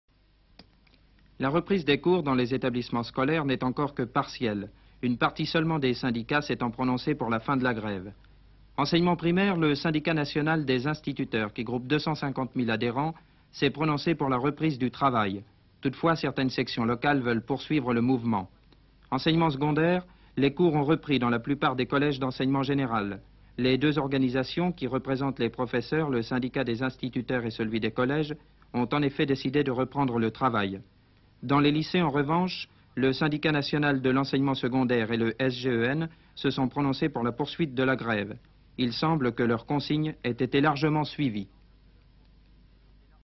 Réouverture des écoles
7 juin 1968
Reprise des cours dans les établissements primaires et les collèges d'enseignement général. politique; archive television; archive tv; ina; inna; Institut National de l'Audiovisuel; french tv Images d'archive INA